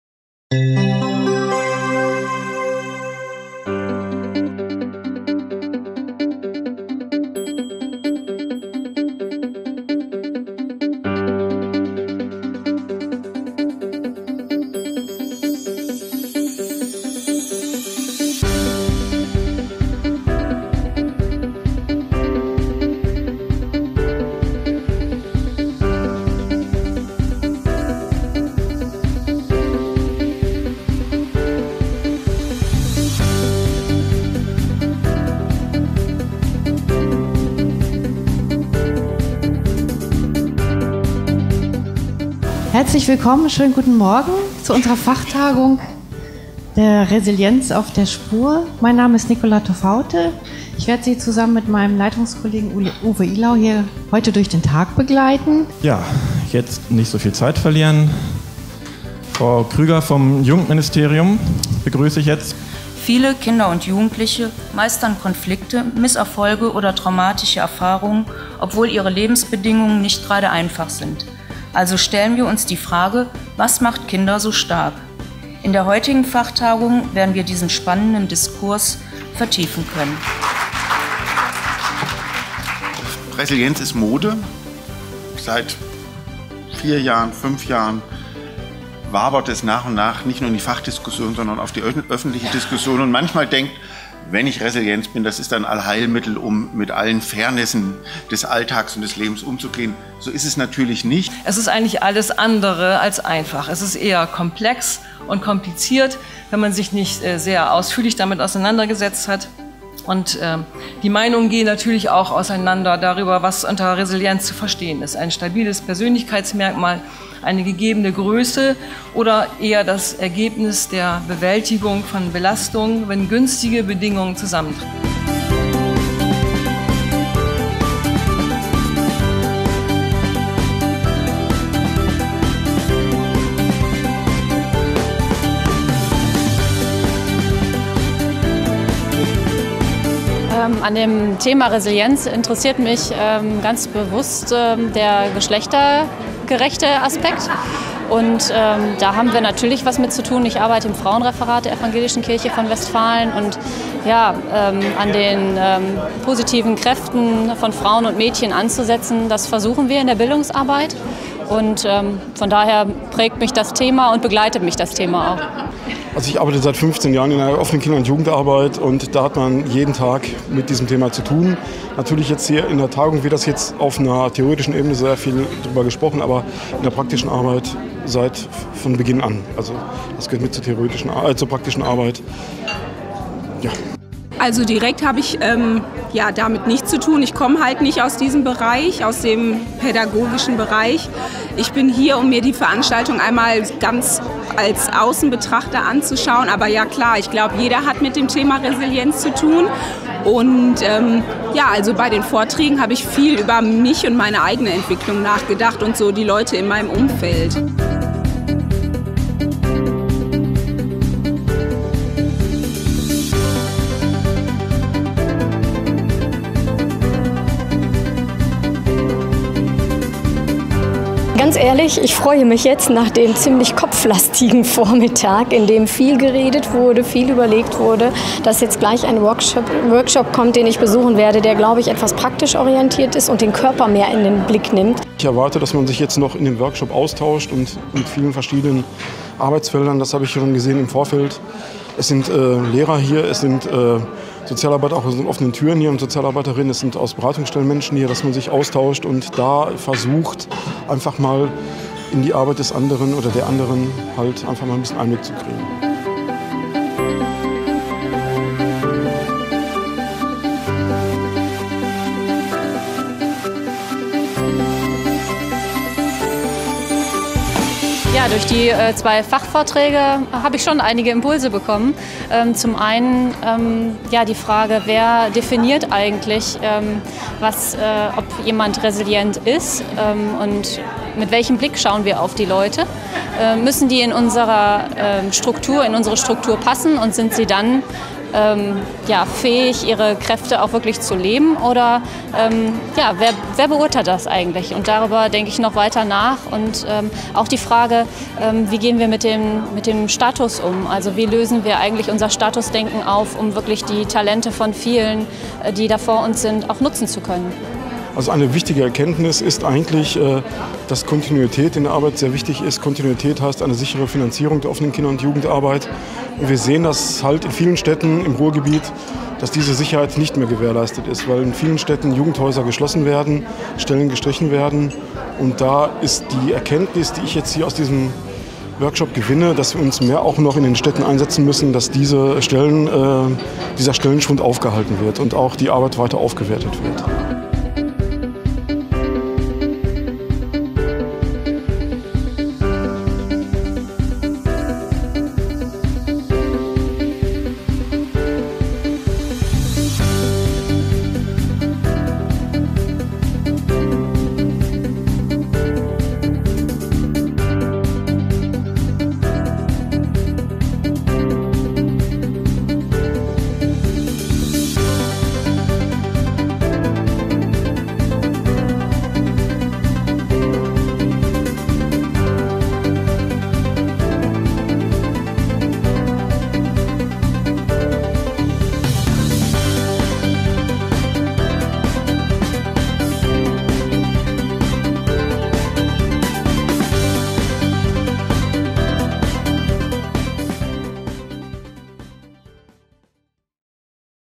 Impressionen der Fachtagung der FUMA Fachstelle Gender NRW am 15. September 2015
Landesweite Fachtagung "Resilienz - Geschlechtsbezogene Schutzfaktoren bei Mädchen_ und Jungen_" der FUMA Fachstelle Gender NRW am 15. September 2015.